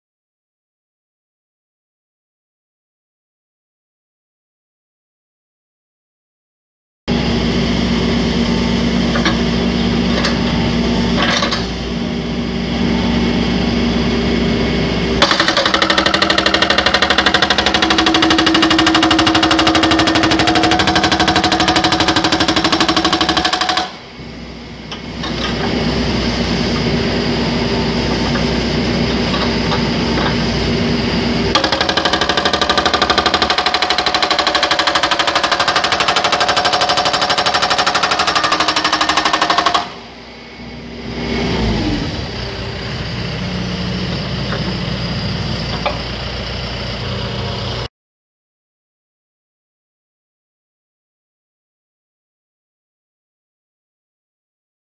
Martillo Hidráulico FURUKAWA KENT F6, Serie: F6-12180
Martillo Hidráulico FURUKAWA KENT F9
Serie: F9-4059
Cople normal ajustable para cualquier equipo
Mantenimiento(e), Incluye Mangueras
Para Retroexcavadora
Recién Importado
Utilice nuestro sistema de separación sin compromiso (Más información)